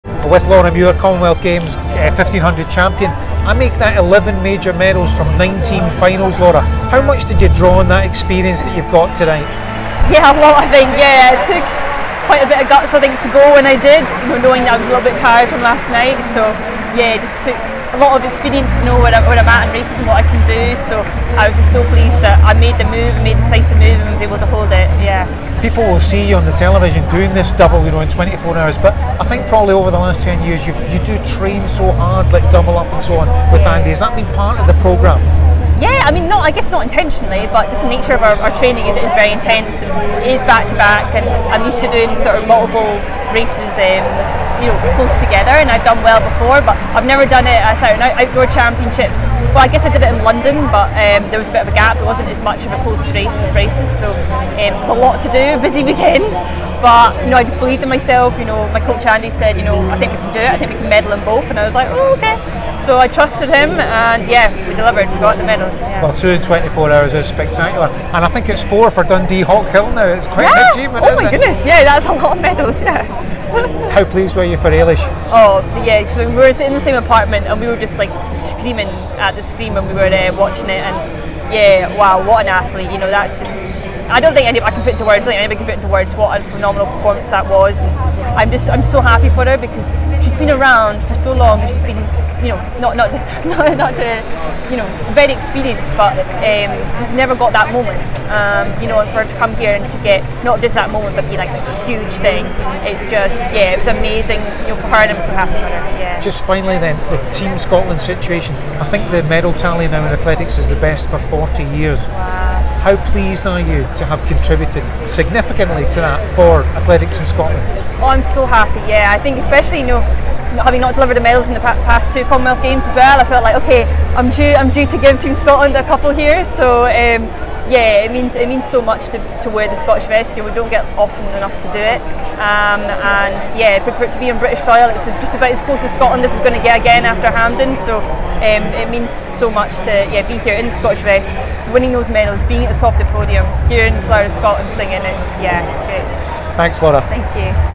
Laura Muir on her 1500m Commonwealth gold medal
Laura talks about her second medal at Birmingham 2022